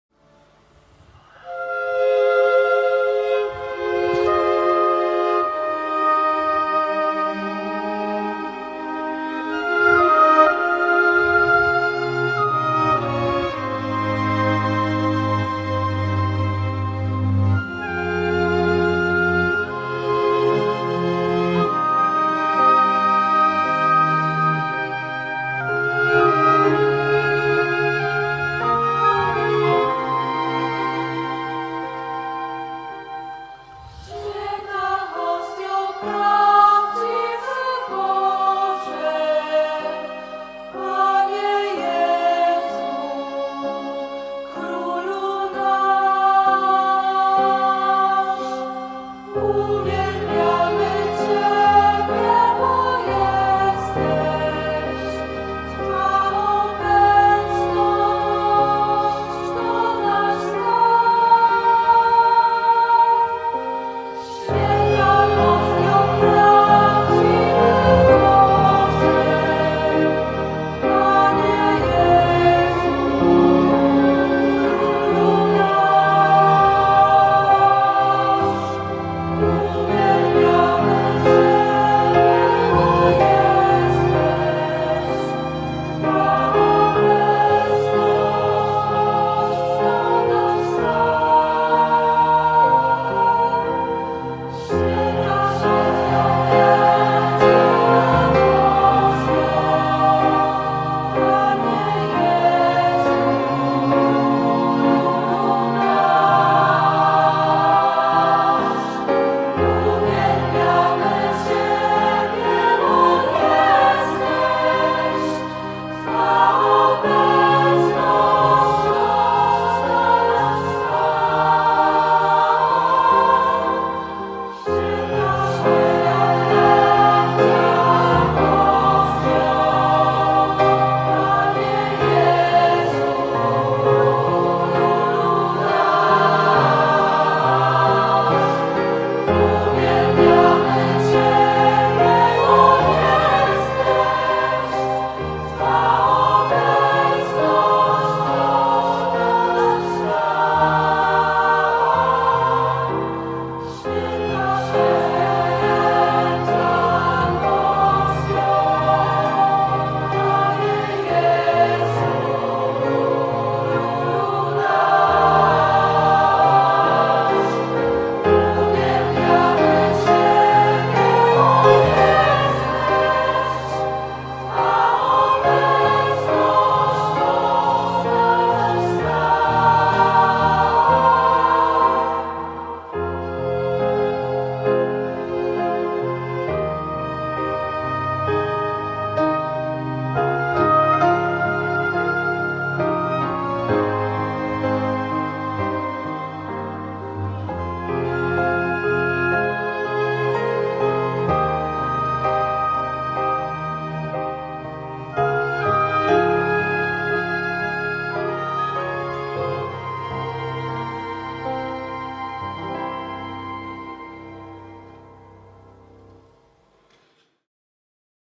Relacja z uroczystości Bożego Ciała w Pelplinie [4K]
Relacja z uroczystości Bożego Ciała w Pelplinie.
W czwartek, 16 czerwca 2022 r. Biskup Pelpliński Ryszard Kasyna przewodniczył uroczystości Najświętszego Ciała i Krwi Pańskiej w bazylice katedralnej w Pelplinie.